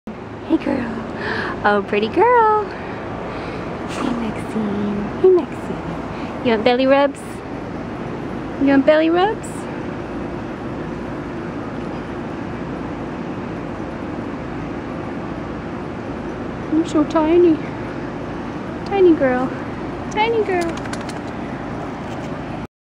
Maxine
Maxine is an adoptable dog from the Humane Society of Alabama.  For more information Maxine and other adoptable animals